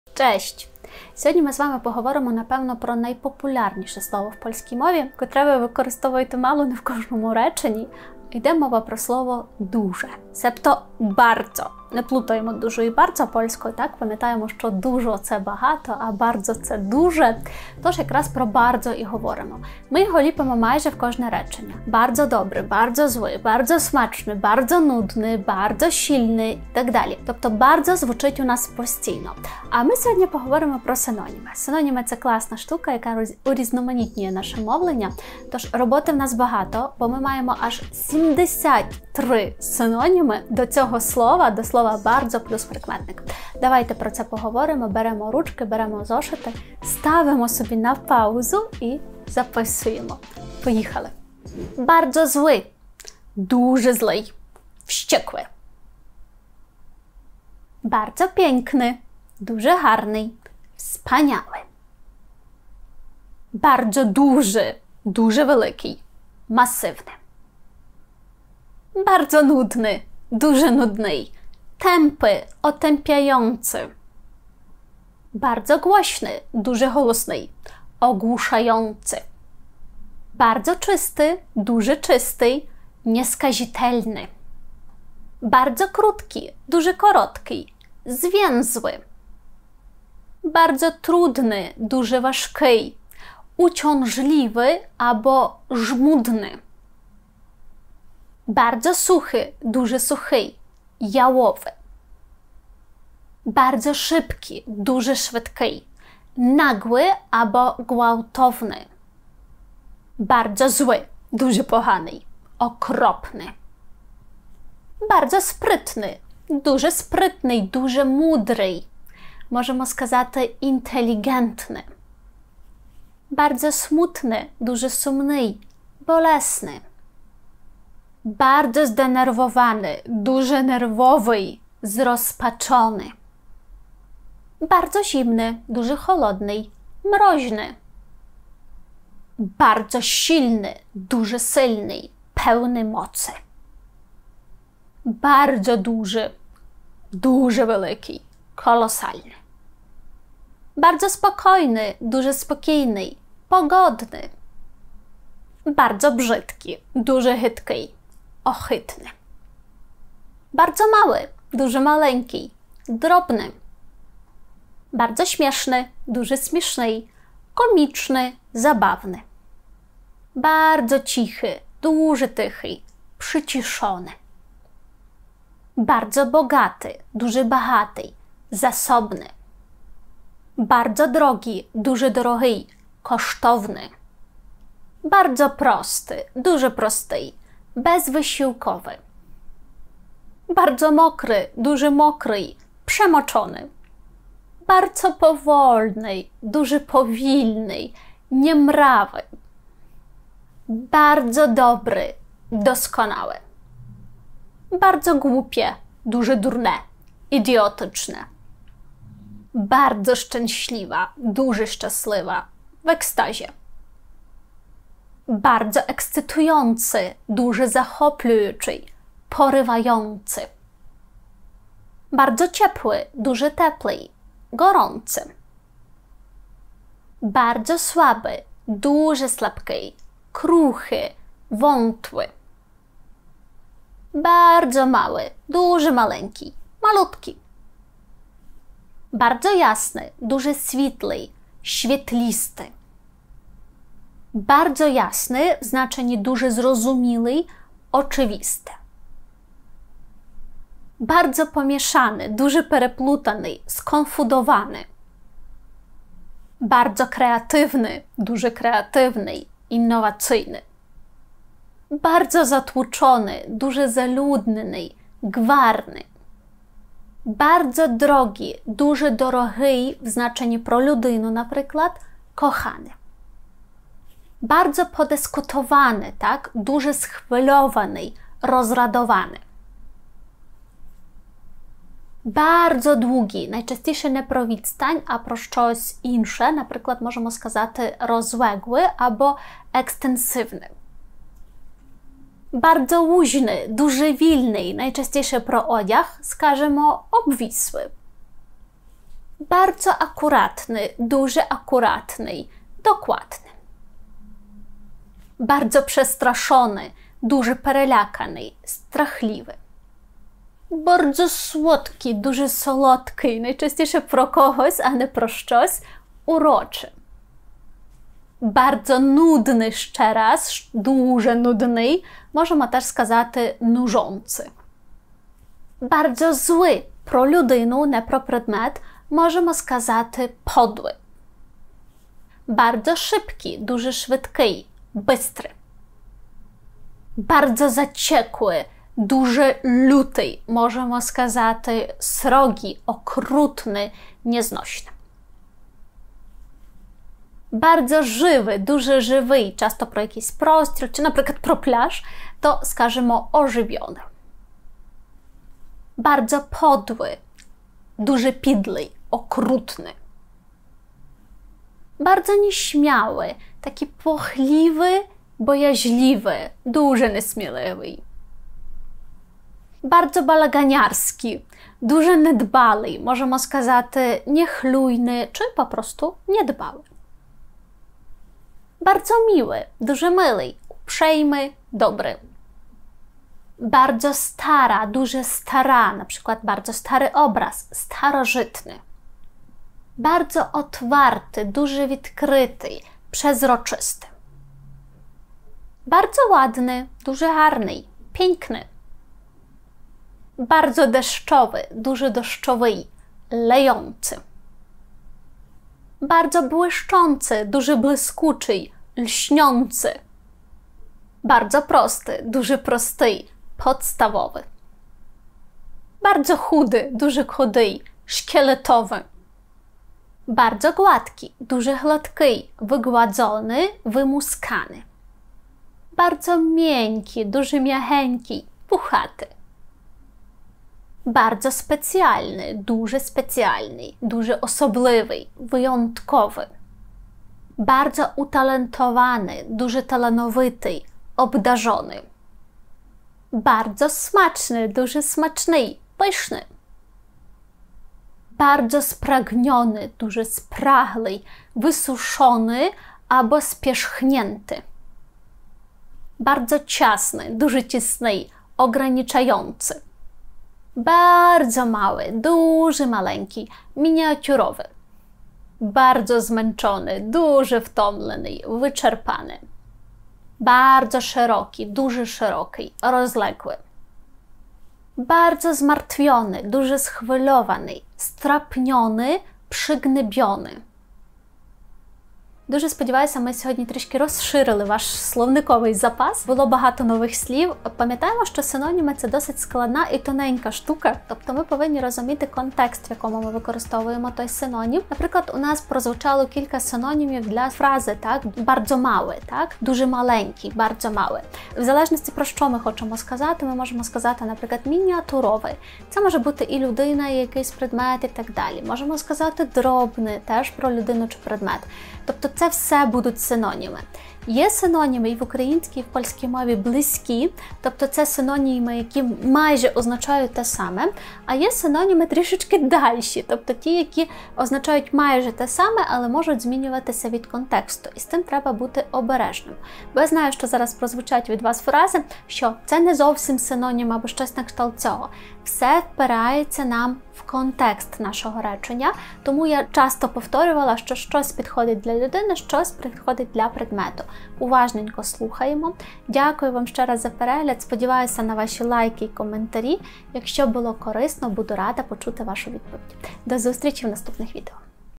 Не кажи більше bardzo польською!
Хоче вивчити польську? Приходь до мене на курси!

Слово bardzo точно входить в ТОП-100 найвживаніших польських слів. Але ж ми усі хочемо розмовляти польською гарно, вміло підбирати слова та фрази. В цьому нам дуже допомагають синоніми. Якщо Ти хочеш розширити свій словниковий запас польською, то це відео саме для Тебе!